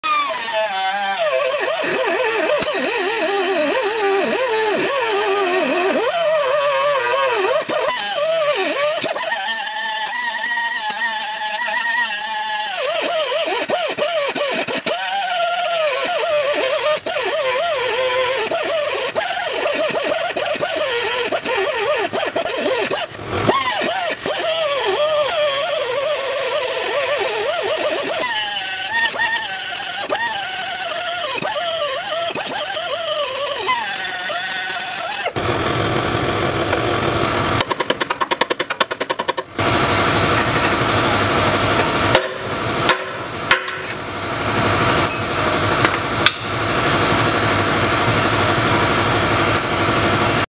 Harbor Freight air cut tool review
See the Harbor Freight Central Pneumatic air cut off tool in action. It does not due well but for only $20 it will work if you need it to